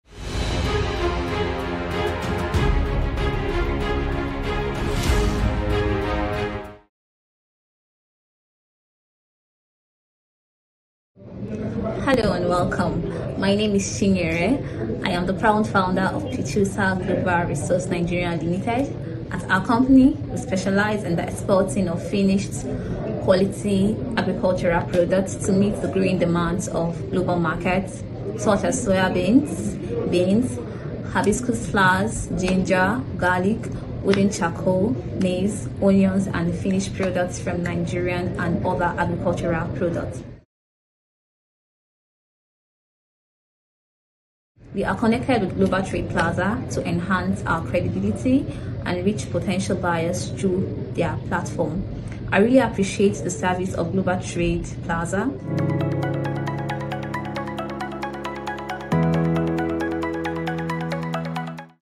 Client has Succeeded In their Plans With the Help of GTP
Ms. Chinyere is the owner of a Company which is based in Nigeria. The company manufactures top-tier quality nuts, agro products, charcoal, and other agricultural products across the globe such as soybeans, maize, dry cassava, garlic, dry split ginger, and much more. To expand her company, Ms. Chinyere has collaborated with Global Trade Plaza, and within record time, they have received many verified buyers for her high-quality products. She greatly appreciates the services of GTP and its management and is looking forward to working with us again.

𝗝𝗼𝗶𝗻 𝗚𝗹𝗼𝗯𝗮𝗹 𝗧𝗿𝗮𝗱𝗲 𝗣𝗹𝗮𝘇𝗮 𝘁𝗼𝗱𝗮𝘆 𝗼𝗿 𝗴𝗶𝘃𝗲 𝘂𝘀 𝗮 𝗰𝗮𝗹𝗹 𝗮𝘁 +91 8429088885.